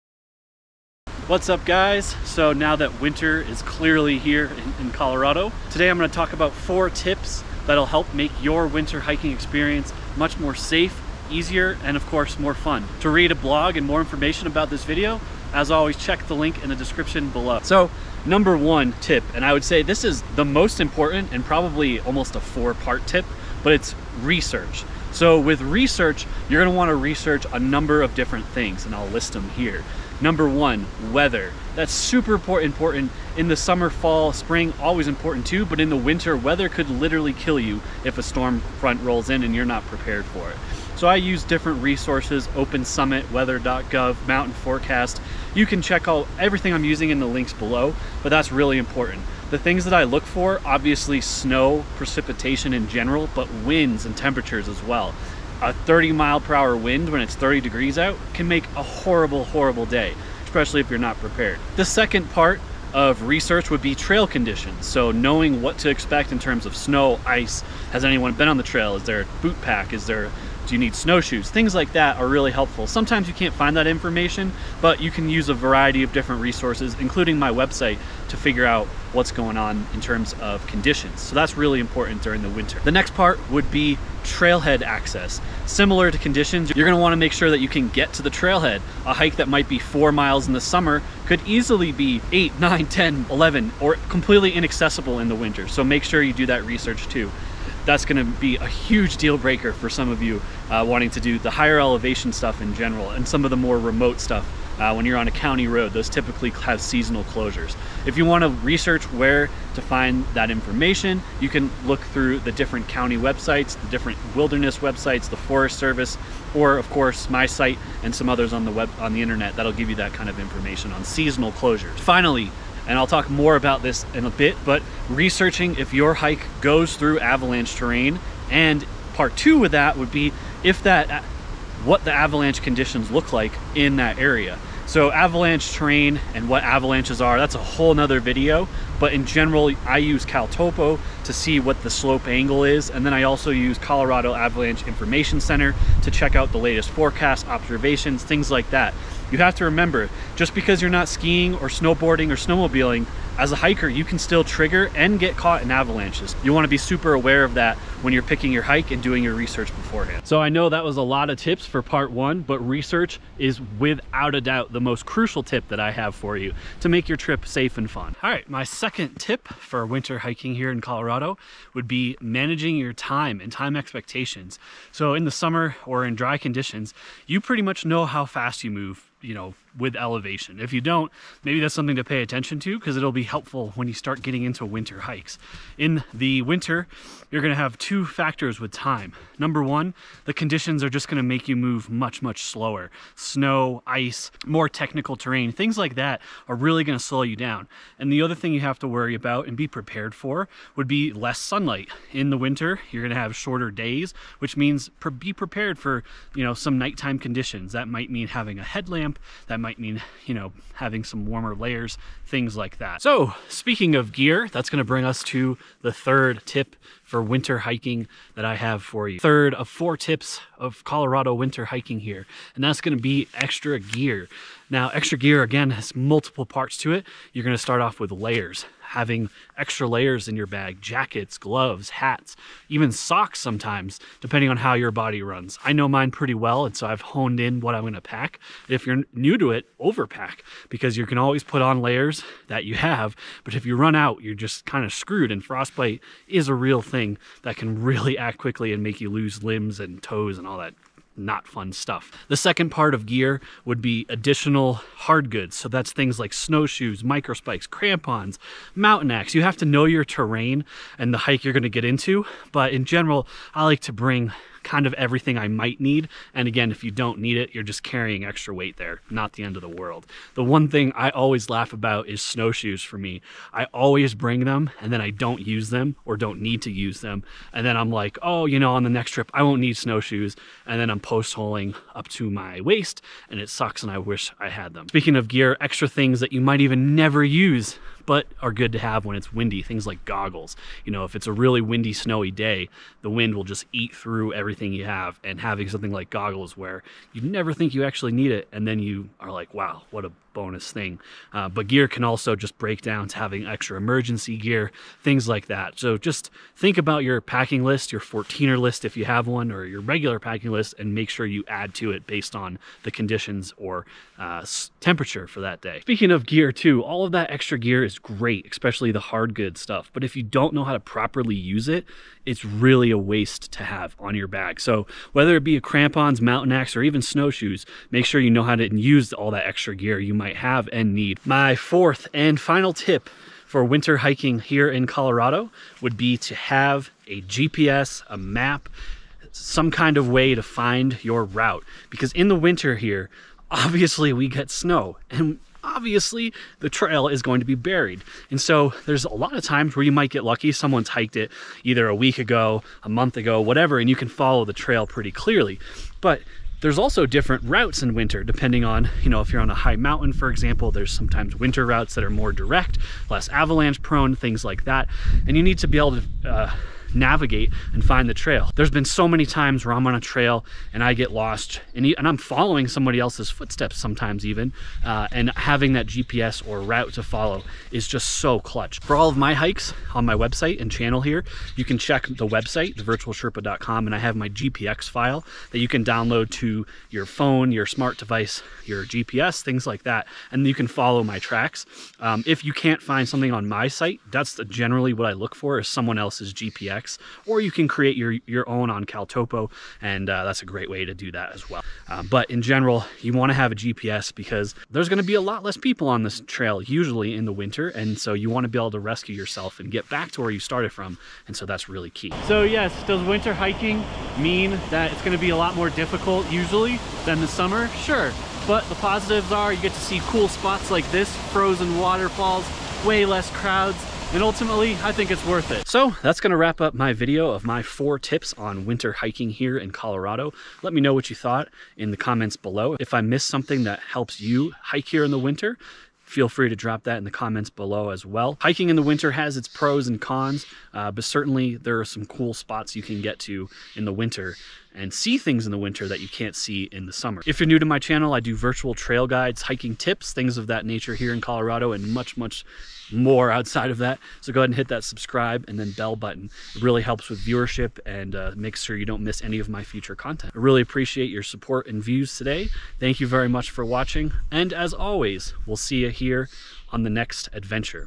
FOUR Colorado WINTER Hiking Tips & Advice
Winter hiking in Colorado can be tough, in this video I discuss my top 4 Colorado Winter Hiking Tips to make it a little less difficult. Ideally, this video will help you feel better prepared, more safe and have MORE FUN on your next Colorado winter hike.

Read a more detailed blog with all of the resources I mentioned in the video on my

Filming Equipment

GoPro Max
Selfie Stick
Joby Tripod

0:00 - 0:15 Intro
0:15 - 2:47 Tip 1: Research
2:48 - 3:32 Tip 2: Time Management
3:33 - 5:30 Tip 3: Packing Extra Gear
5:31 - 7:47 Tip 4 Have GPS